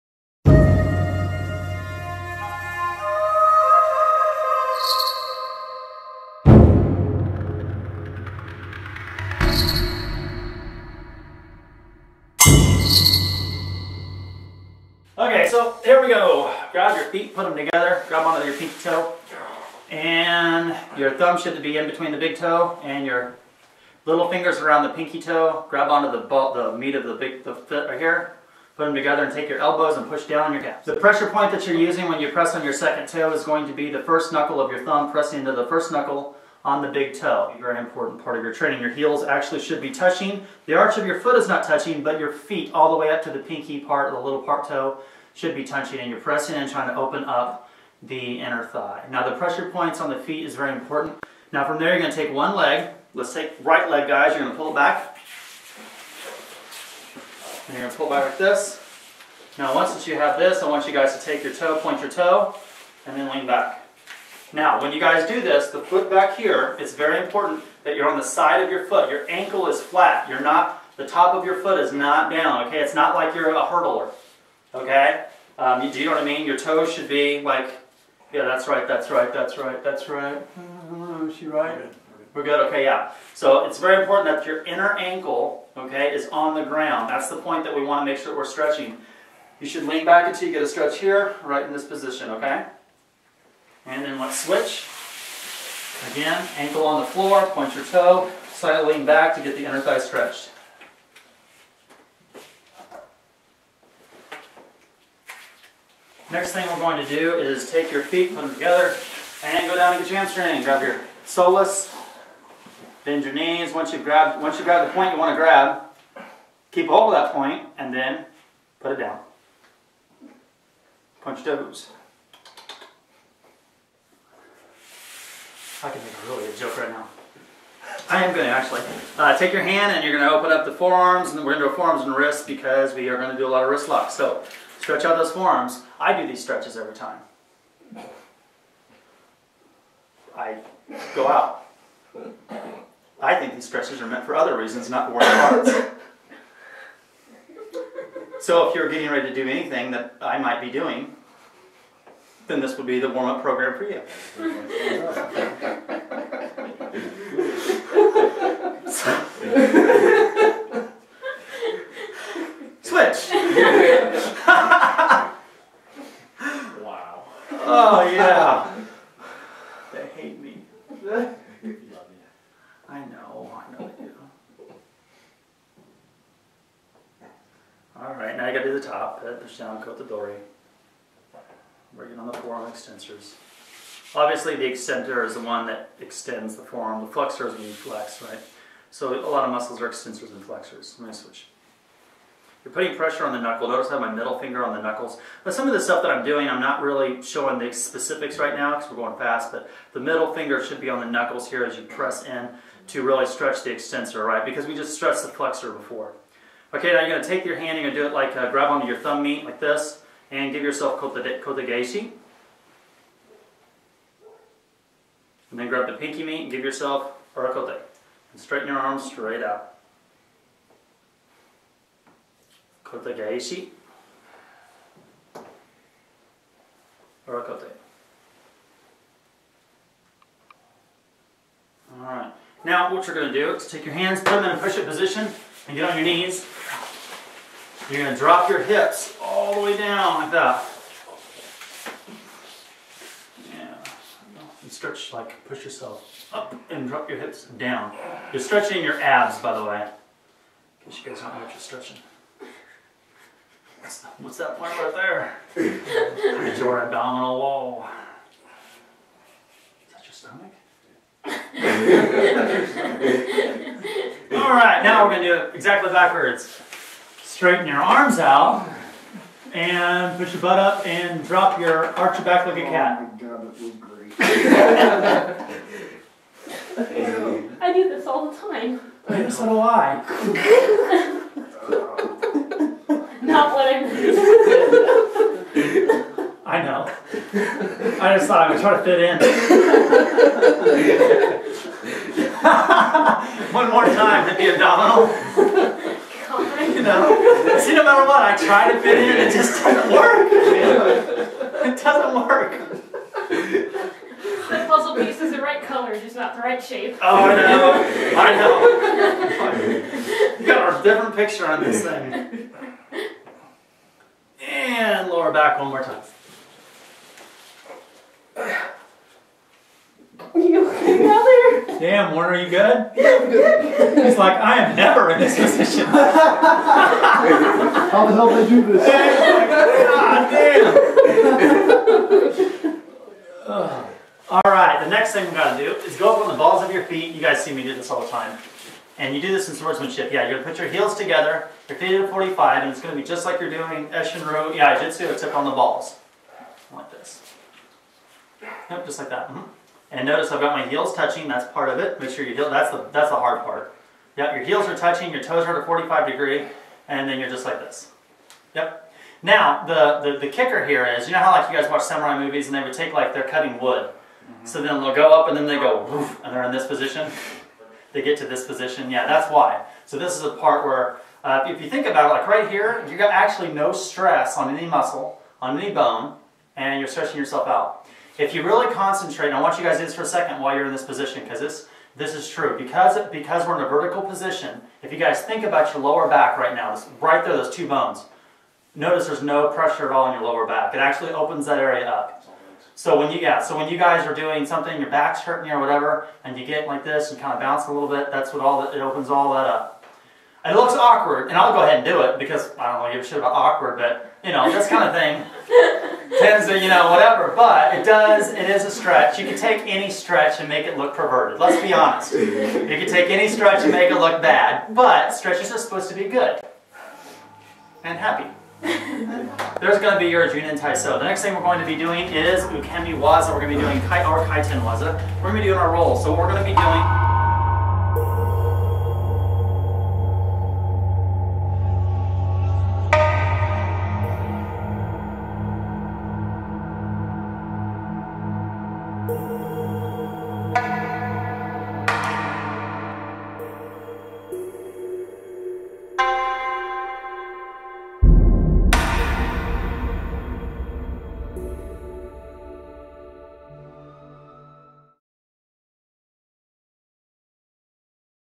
How To Increase Your Flexibility For Martial Arts | Combat Conditioning, Hojo Undo, Junan Taiso
In this video I teach how to better your flexibility for marital arts training!

This lesson is directly for the practitioners of the ancient Japanese koryu martial arts of the ninja and samurai, such as ninjutsu (ninpo) and bujutsu (budo).

Follow Soke Anshu on Facebook!